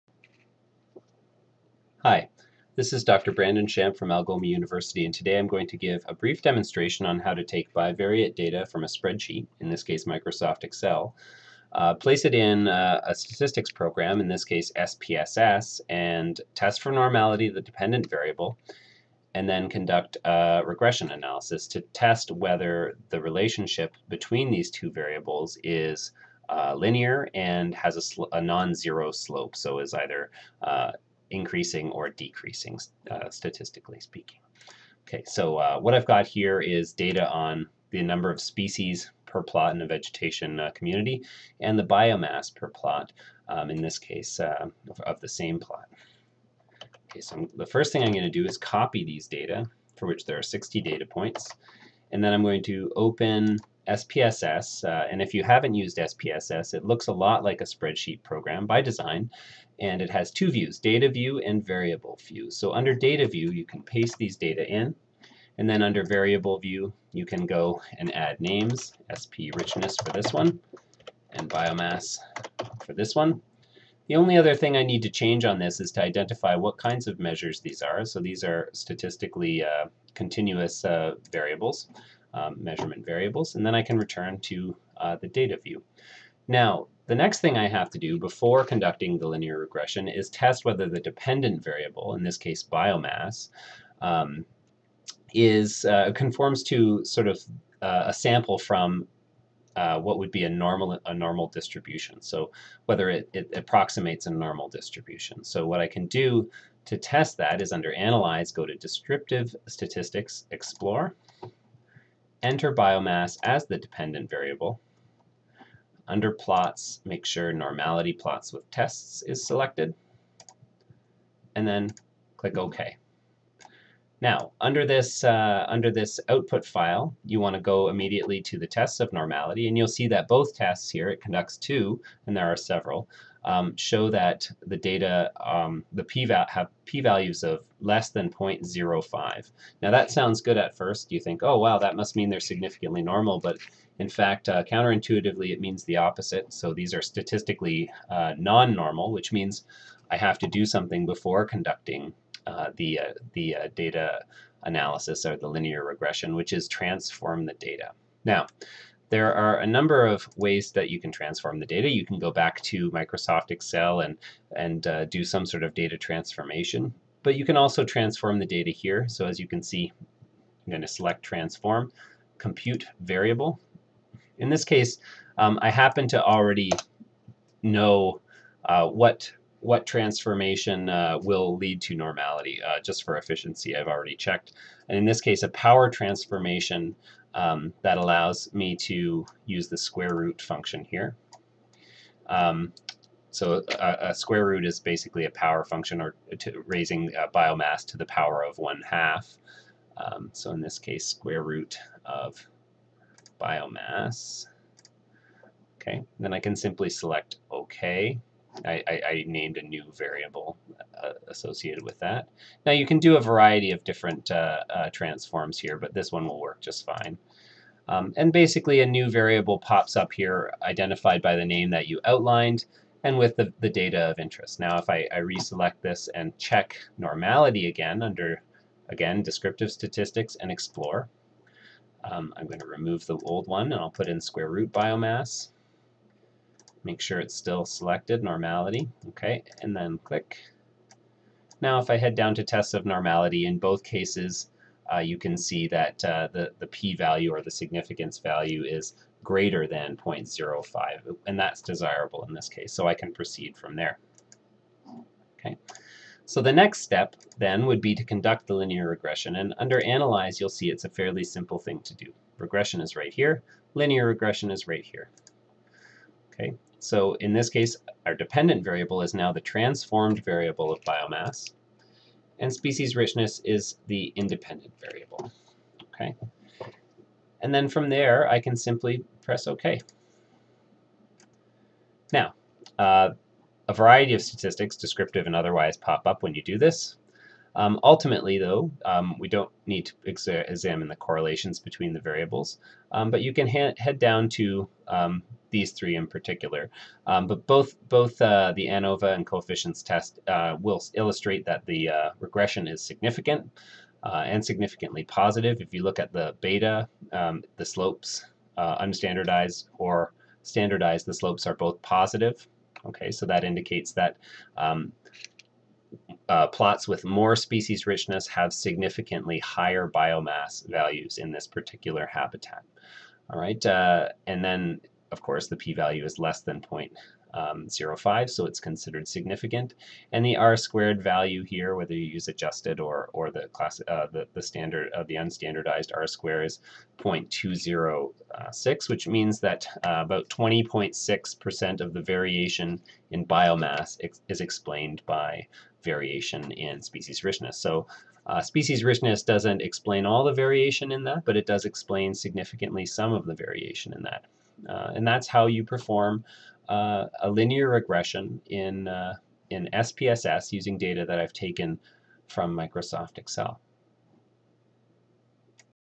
Data Transformation and Linear Regression in SPSS
This is a short video explaining how to take bi-variate data (an X and Y variable), place them in the statistics program SPSS (version 21), and assess normality of the dependent variable, transform that variable to improve normality, and then run a linear regression analysis to test whether the dependent variable (Y) changes in a linear pattern with variation in the independent variable (X) and whether the slope of that relationship differs significantly from zero.